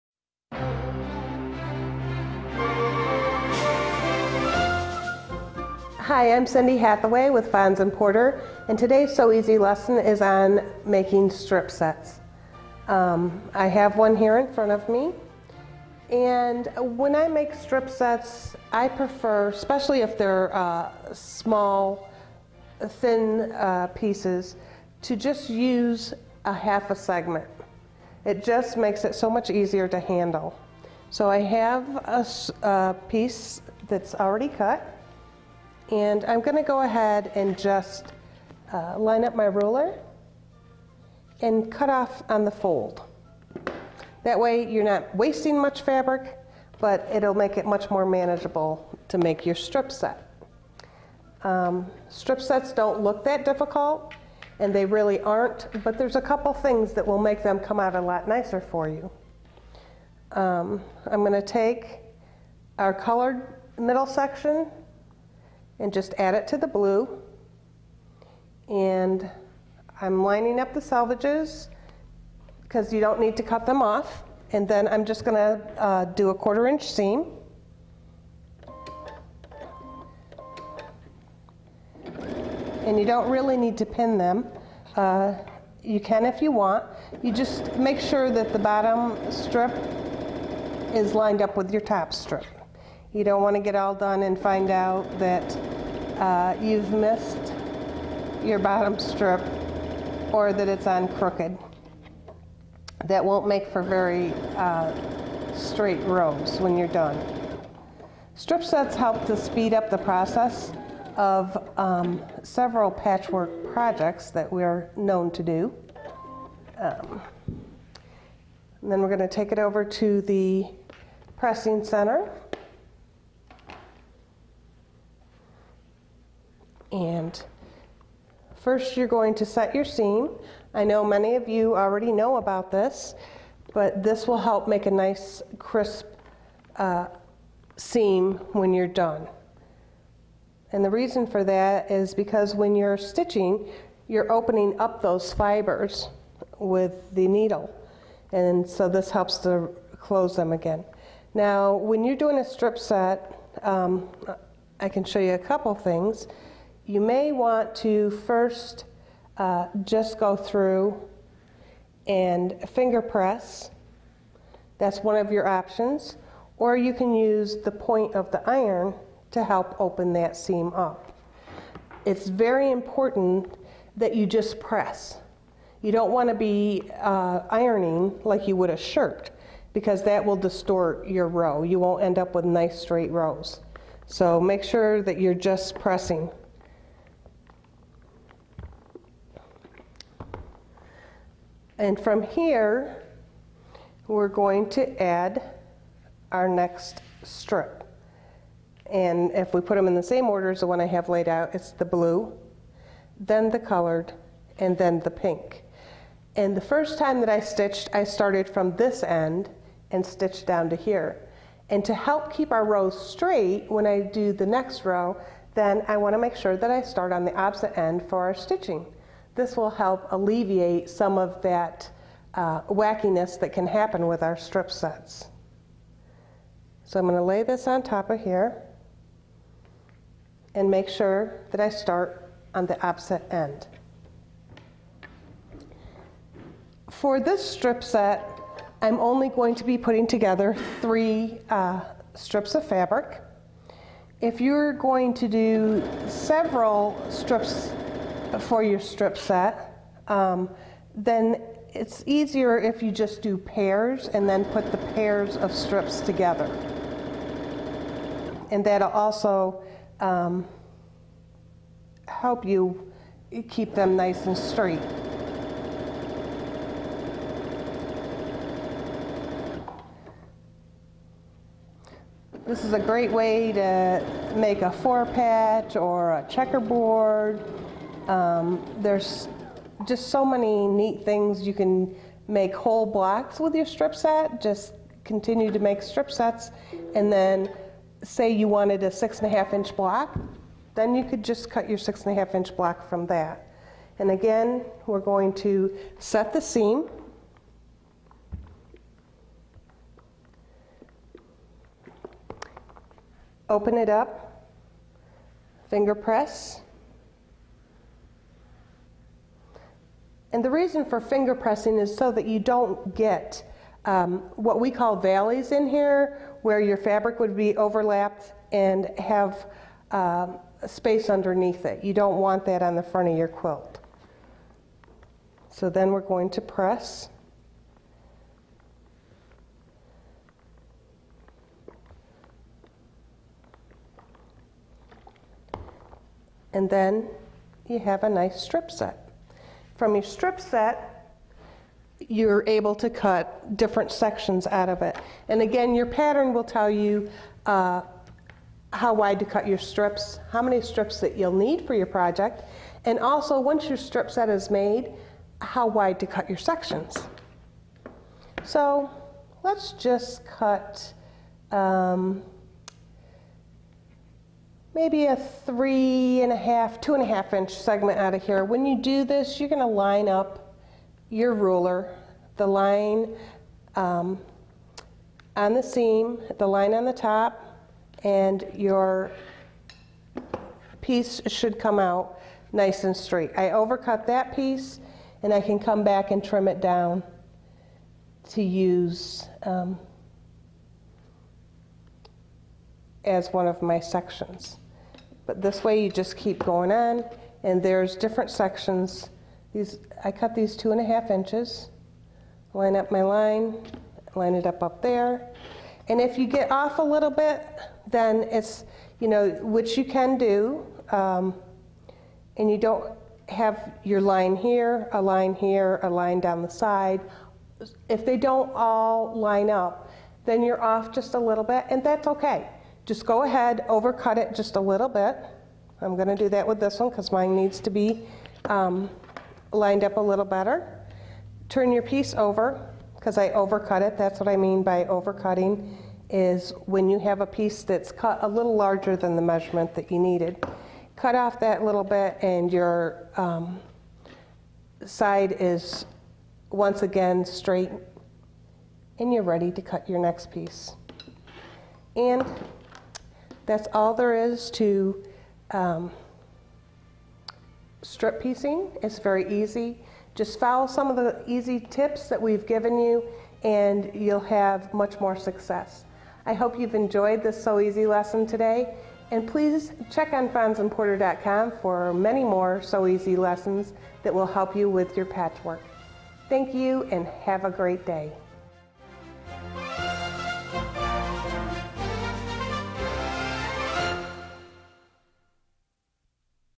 Sew Easy Lesson: How to make Strip Sets
The basis of many quilt instructions, strip sets make piecing go quickly and efficiently. Learn how to make them in this video.
 A strip set is a group of strips that have been sewn together lengthwise in a particular sequence. Completed strip sets are later cut into smaller segments to use as blocks or as portions of blocks.  For printed instructions of this video, visit FonsandPorter.com.

Fons & Porter 8" X 14" Basic Rotary Cutting Ruler